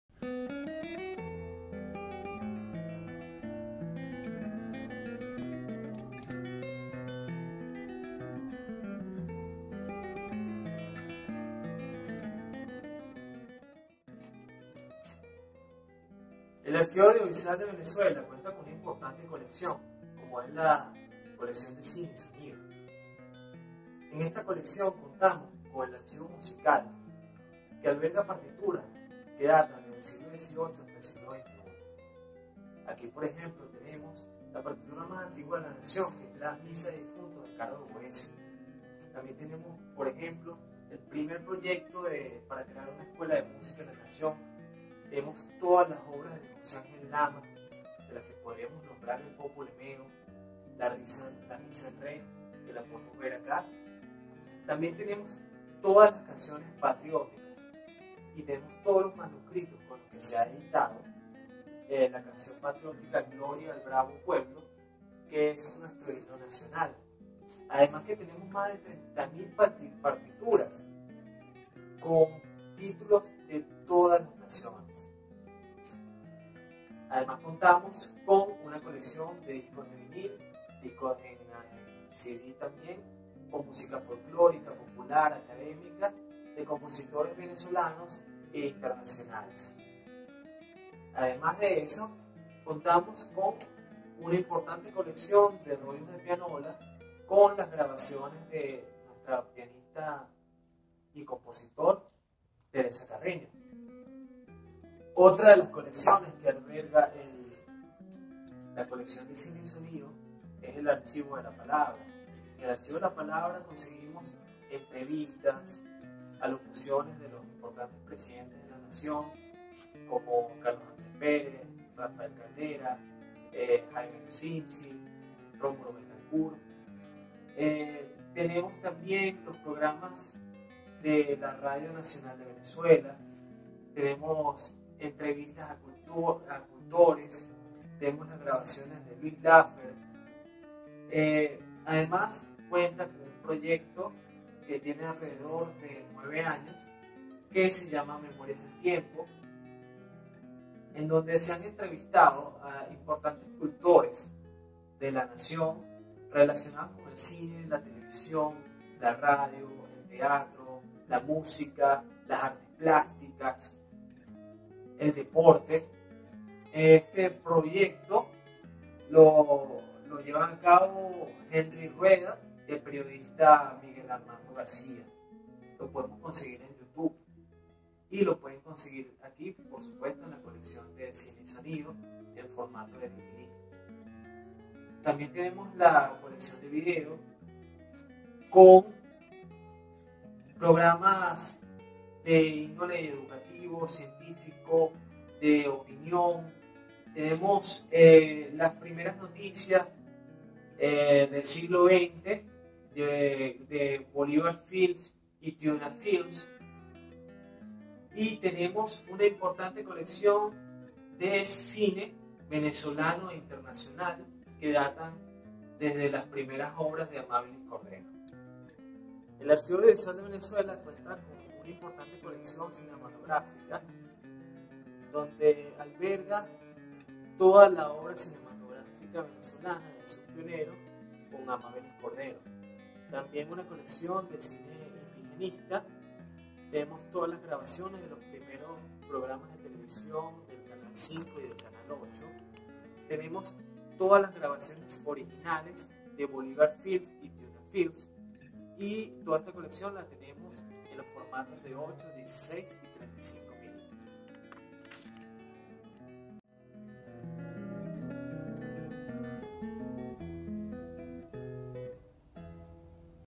Descubre la Colección de Cine y Sonido de la Biblioteca Nacional de Venezuela
En nuestro 187° aniversario te ofrecemos un recorrido por la Colección de Sonido y Cine, referente para el estudio de la historia musical, radial, televisiva y cinematográfica de Venezuela. Adrián Sánchez, musicólogo del archivo, te guía en esta oportunidad.
Estamos